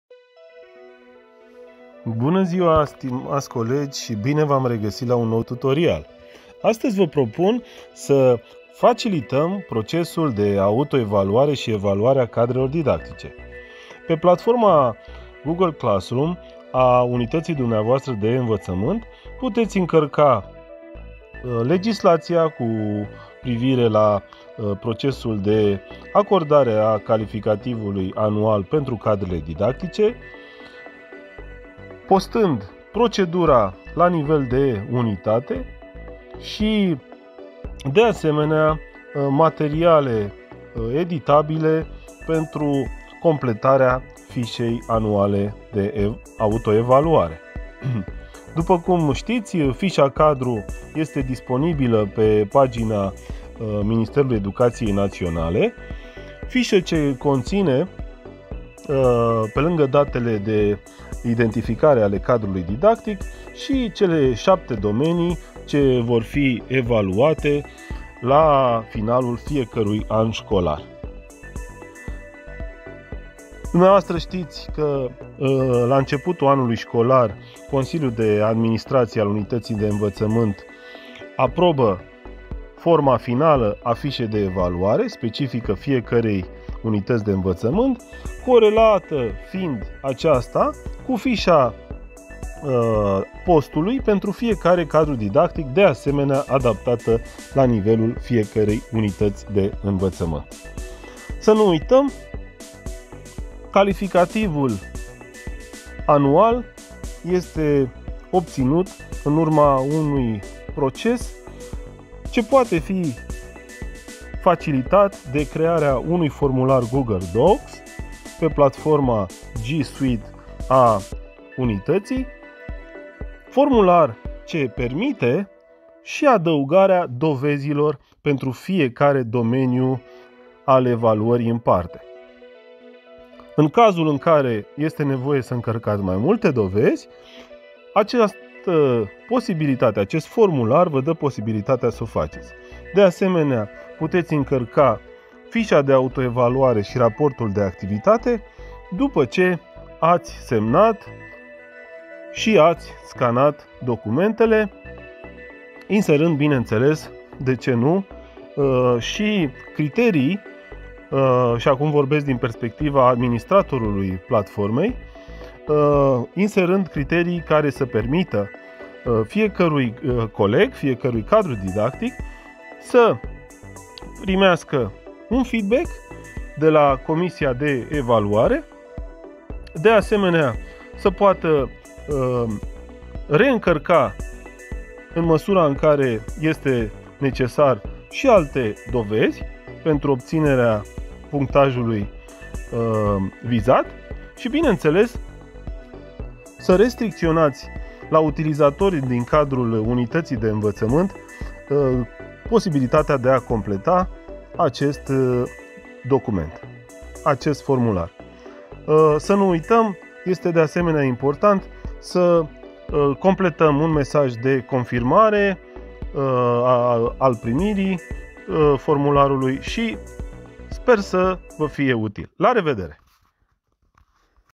Fișa de evaluare anuală a cadrelor didactice
Scopul resursei este de a susține dezvoltarea de competențe digitale a cadrelor didactice și în special a directorilor unităților de învățământ urmărind eficientizarea procesului de colectare a rapoartelor de activitate și a fișelor de autoevaluare anuale pentru cadrele didactice incluzând posibilitatea adăugării de dovezi necesare membrilor comisiei.
Cuvinte cheie: evaluare, autoevaluare cadre didactice.